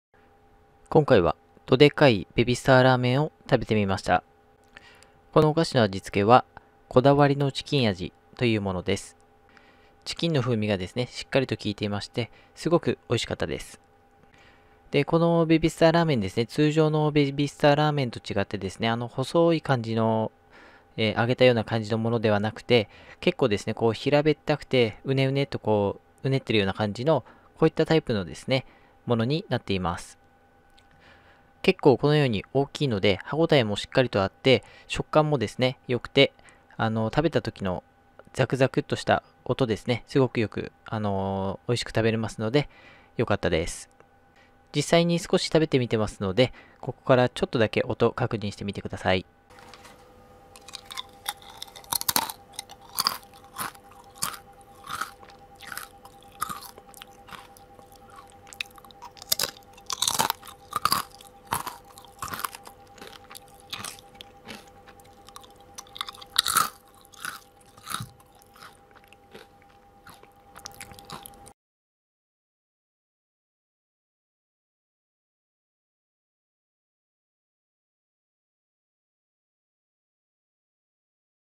【おやつカンパニー】 ベビースタードデカイラーメンHikakinプロデュース こだわりチキン味 Baby Star Dodekai Ramen Hikakin Produce
ベビースターラーメンとても人気のあるスナック菓子のひとつです!
そんなベビースターラーメンが、 YouTuber HIKAKIN プロデュースでベビースタードデカイラーメンという形で販売していました。
味はこだわりのチキン味ということです。

チキンのコクがしっかりとありとても美味しかったです。
それほどしょっぱすぎず、噛んだ時の食感もよくザクザクとした音も良かったです。
ビールやお酒のおつまみにも凄く良さそうなので、お酒が好きな方も一度食べてみてください。

【動画のものとは違いますがベビースタードデカイラーメン別の味であれば、アマゾンでも購入できます】
■　おやつカンパニー ベビースタードデカイラーメン(チキン) 74g×12個

■　おやつカンパニー ベビースタードデカイ焼そばマヨソース味 73g×12袋
↑　クリックすると移動します

Baby Star Ramen is one of the most popular snacks!
Such baby star ramen was produced by YouTuber HIKAKIN and sold in the form of baby stard dekai ramen.
The taste is a particular chicken taste.

The richness of the chicken was solid and it was very delicious.
It wasn't too salty, it had a nice texture when chewed, and the crispy sound was good.
It seems to be very good as a snack for beer and sake, so even if you like alcohol, please try it once.